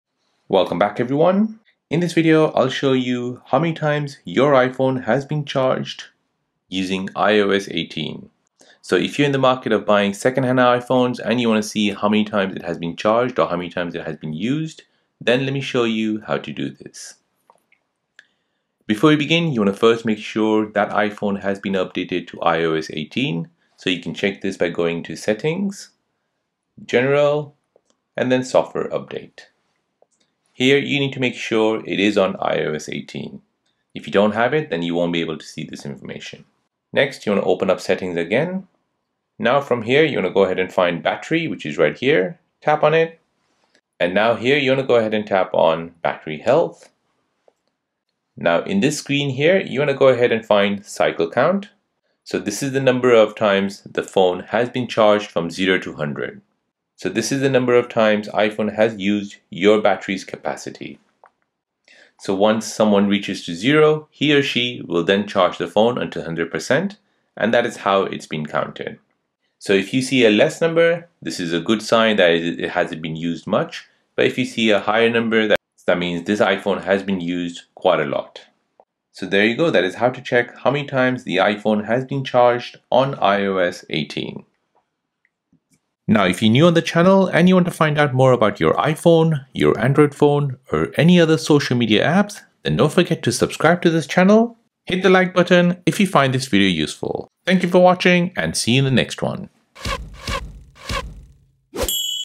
How to Check How many Times iPhone has been Charged | Battery Life Check iOS 18 iPhone 15 above!
00:00 Intro
00:10 Like or no Like
00:20 How to Check How many Times iPhone has been Charged